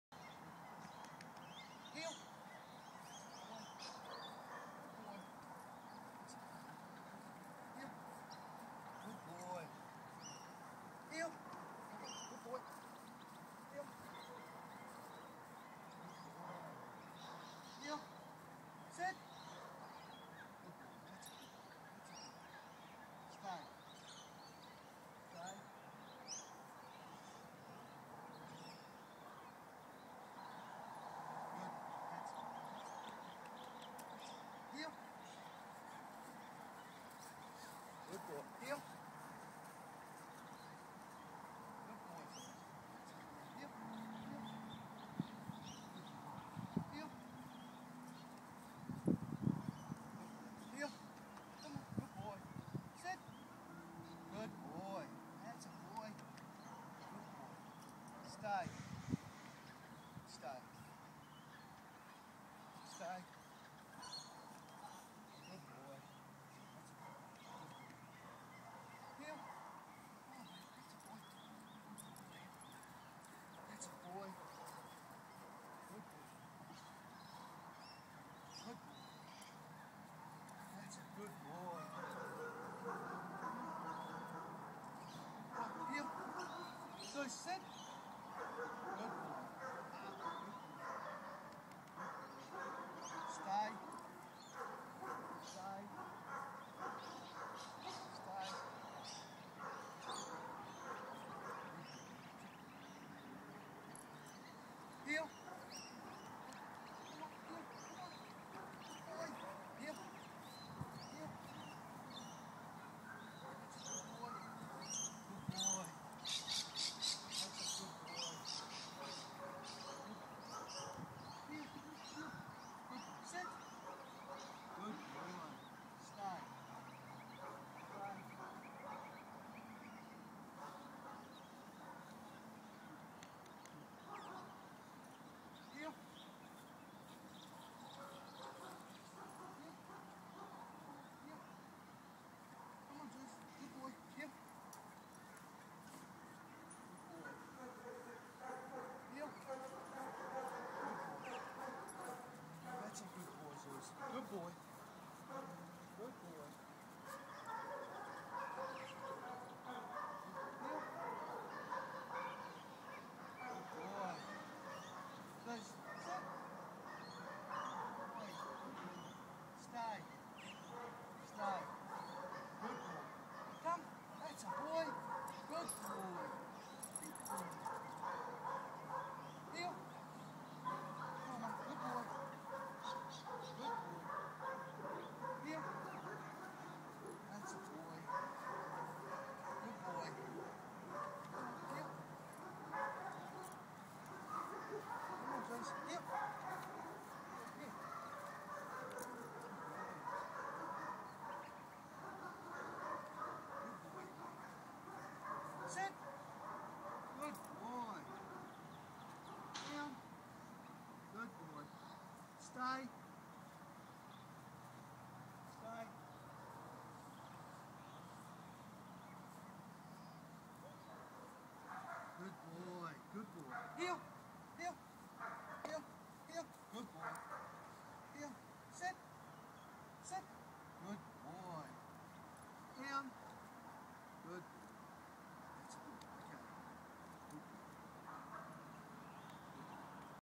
Doberman Dog Training Zues Takes On K9 Master Class
Zues the Doberman is instructed by Scott Donald through the SOX Method developed by the K9 Master Class. This Doberman responded very well to the training and can now Sit, Stay, Heel and Come.

to learn more about training your Doberman.